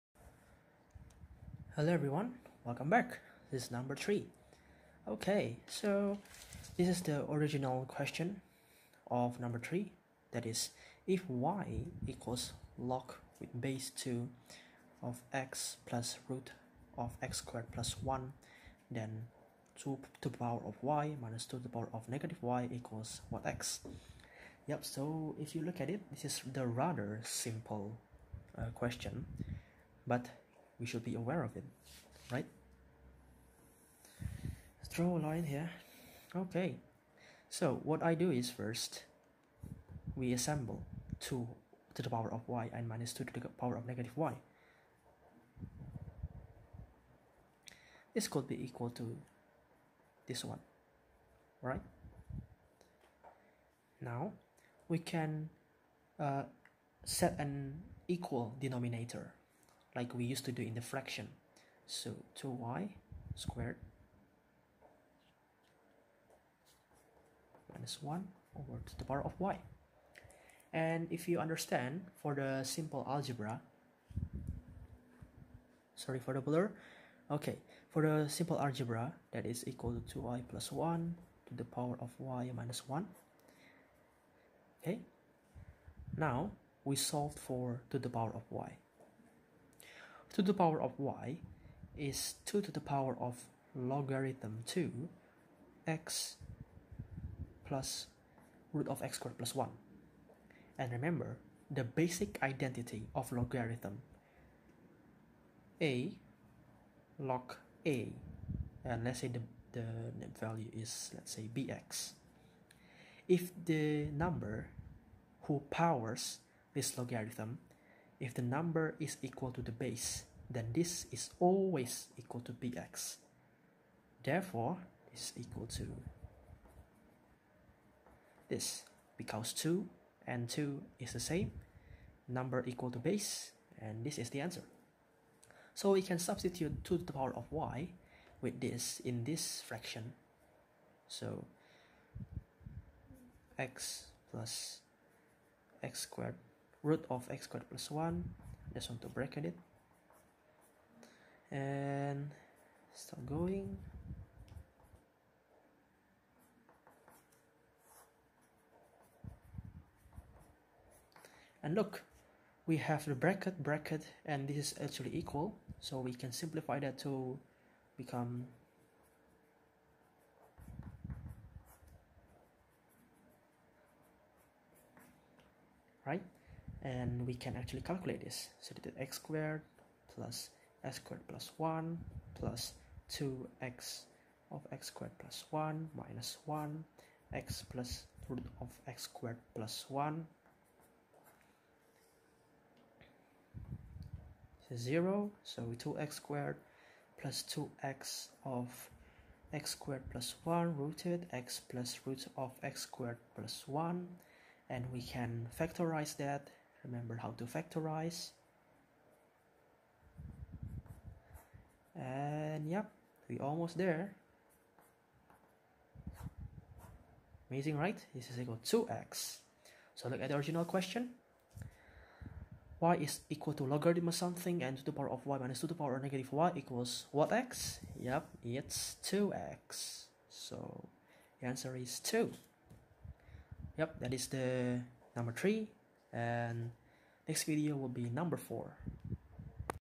MONBUKAGAKUSHO 2015 - MATHS B Part. 1 No (3) Undergraduate Students
Monbukagakusho video solution for 2015 Paper of Mathematics B. Stay toon for more awesome solution videos !!.